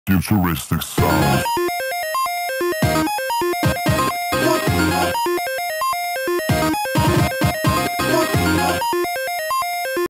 Coming Soon
Blindfire Coming soon with their latest production.. "Battery"

Stay updated on news!

Animation made by Anthony Breslin.